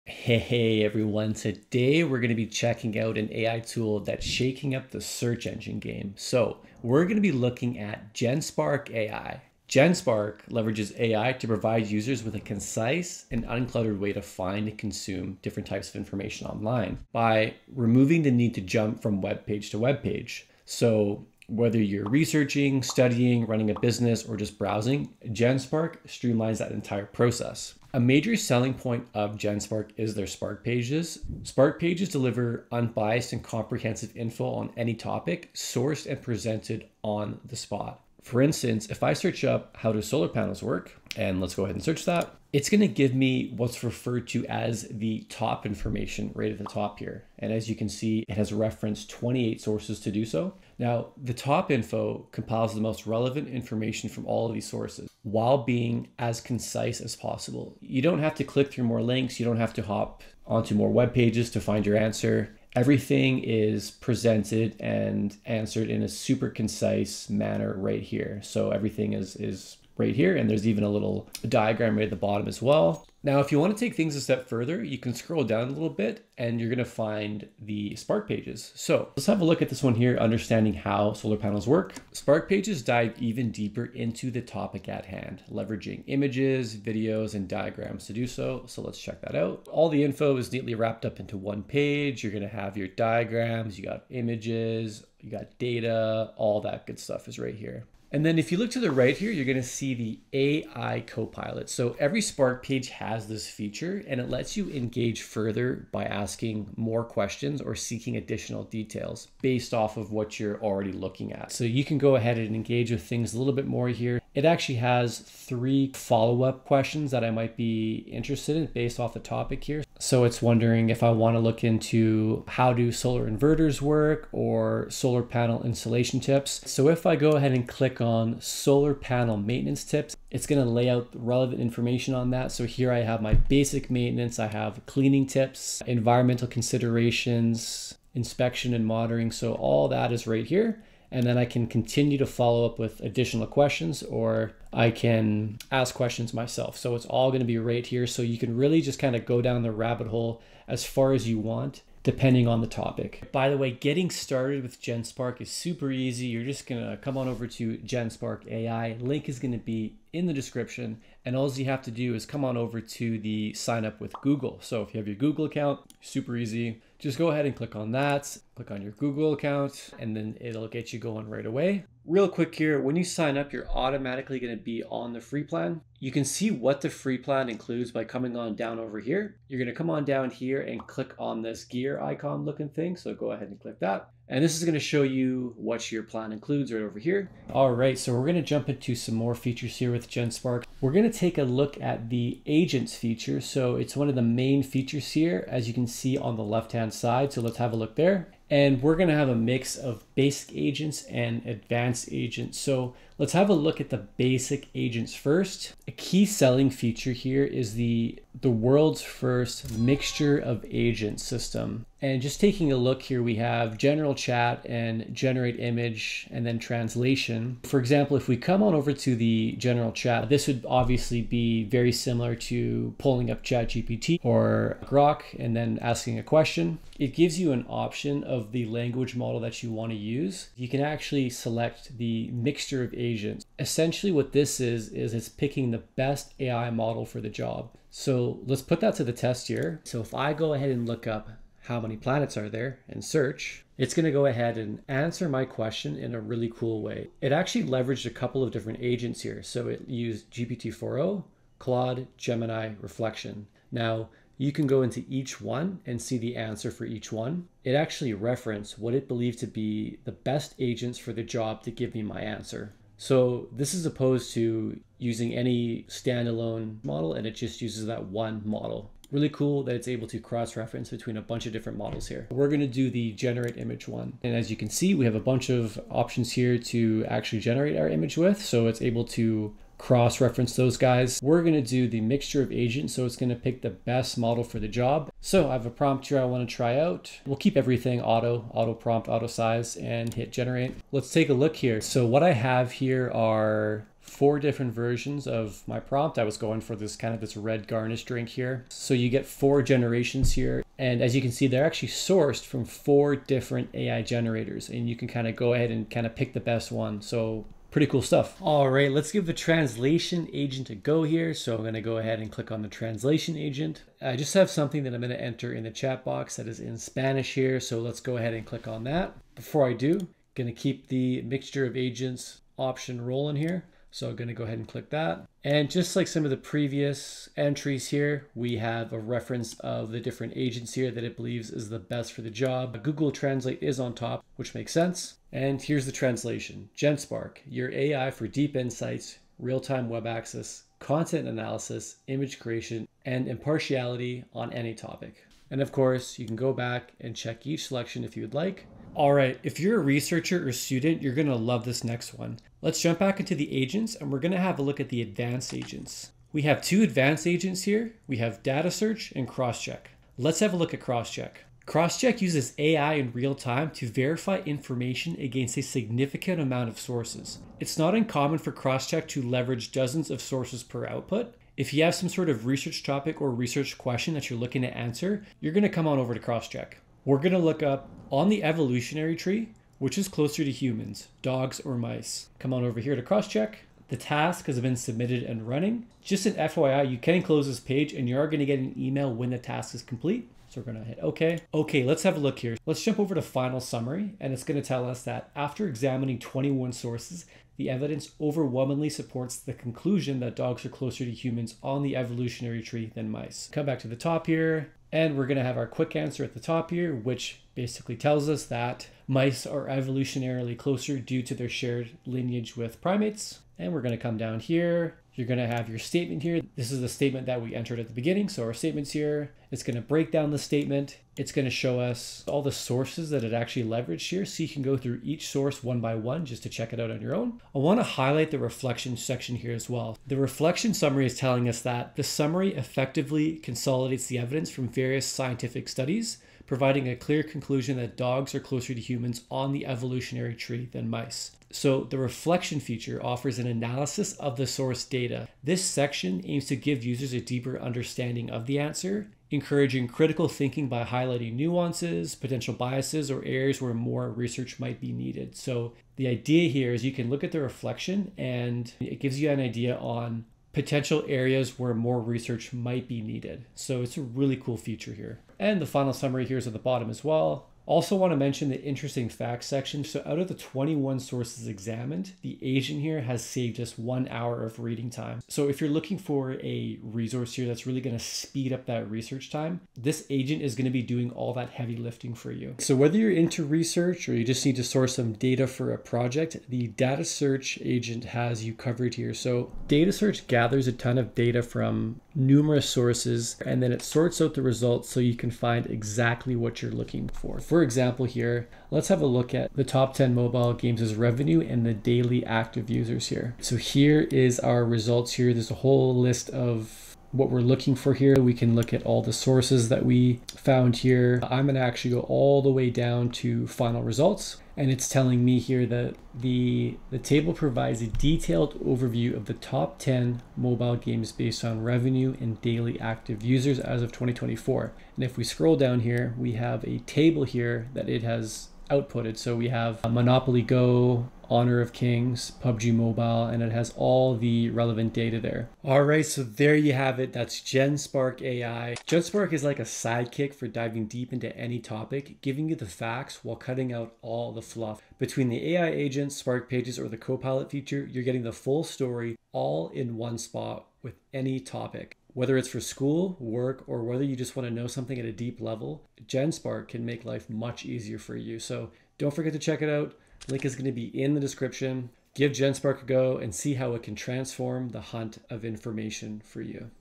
Genspark AI: Free AI Search Engine (Now with Intelligent Agents!)
Timestamps

00:00 – Intro & Overview
00:21 – Genspark’s Key Features
01:09 – Spark Pages Demonstration
02:33 – Sign-Up & Free Plan
03:07 – Agents Overview (General Chat, Image Generation, Translation)
06:01 – Cross-Check Agent
08:03 – Data Search Agent
09:19 – Conclusion & Wrap-Up

What is GenSpark & Video Highlights

Genspark is an all-in-one AI platform that consolidates your research, studies, and casual browsing into one streamlined experience. Rather than juggling multiple tabs, Genspark instantly compiles key insights from various sources and displays them in a concise format—complete with AI-driven search, Spark Pages, and specialized Agents to handle everything from translating text to generating images.

AI-Driven Search & Summaries

Spark Pages present unbiased, comprehensive answers on any topic, including relevant diagrams, videos, and additional AI suggestions via the built-in Copilot.

Agents for Every Task

General Chat: A flexible chatbot interface that taps multiple AI models (GPT-4, Claude, etc.) to provide precise answers.

Image Generation: Create visuals using an intelligent “mixture of agents,” ensuring the best possible output.

Translation: Instantly convert text between languages, leveraging the most accurate AI translator.

Cross-Check: Verify information in real time against dozens of sources, perfect for fact-checking and detailed research.

Data Search: Gathers large sets of data from multiple references, presenting them in neat, easy-to-read tables.

Seamless Sign-Up & Free Plan

Get started simply by signing up with Google.

The free plan includes a range of powerful features—see the exact inclusions in your account settings.

Speed & Accuracy: Quickly locate, compare, and verify information without hopping across the web.

Unbiased Insights: Pulls from multiple sources to give you the whole picture.

Time Saved: The Cross-Check and Data Search agents do the heavy lifting—so you don’t have to.

Try Genspark Today

Ready to revolutionize your online searches and research?
Visit Genspark.ai to sign up for free and see how it transforms your information gathering.